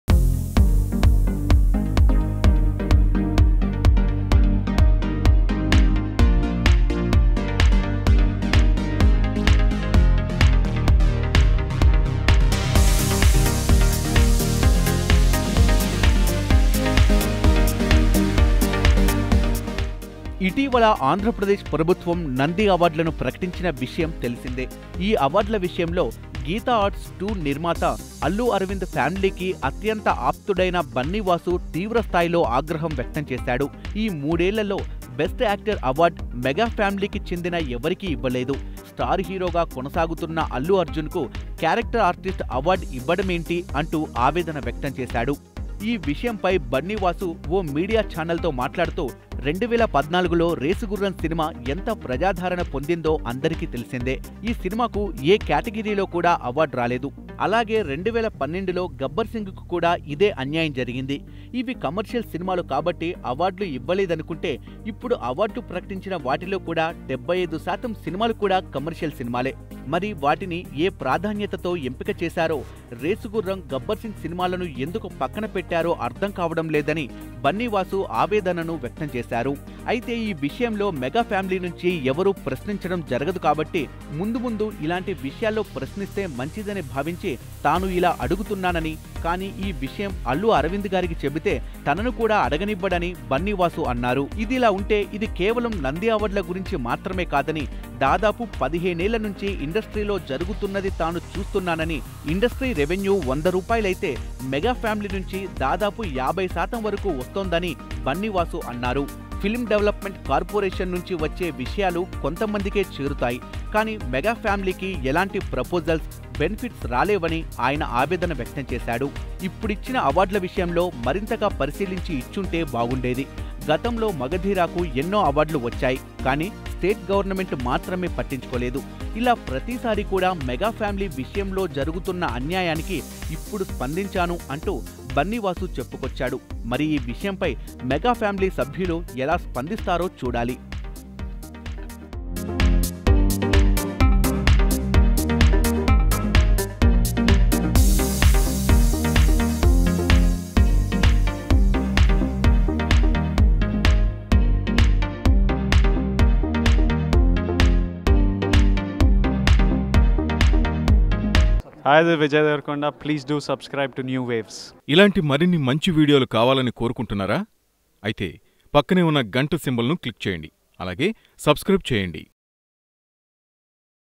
Bunny Vasu Fires On Andhra Pradesh Govt About Nandi Awards | New Waves
after the announcement of nandi awards by the ap govt.. bunny vasu who was unhappy with awards posted on his facebook like this. , "Mega heroes should learn acting from Telugu desam Government such that they will win Nandiawards". And in no time, the post went viral, and the post got deleted in minutes. And then a message reached media people that a fake account posted this, but not real Bunny Vas...